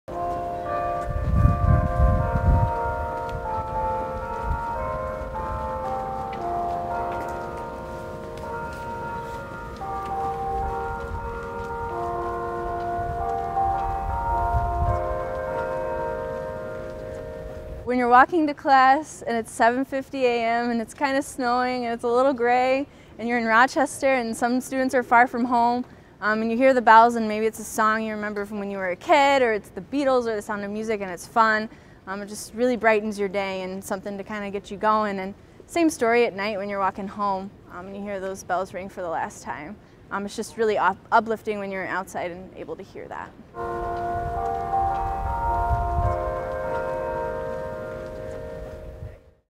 RIT Carillon Bells Ring Again
The sound of the RIT carillon bells was heard on campus for the first time in three years on March 24, 2017. The previous electronic bell system last played in 2014 before breaking. The installation of the new carillon was made possible through the generosity of the Schmitt Foundation, which was recognized by the RIT Student Government before the new bells chimed for the first time.

CAMPUSLIFE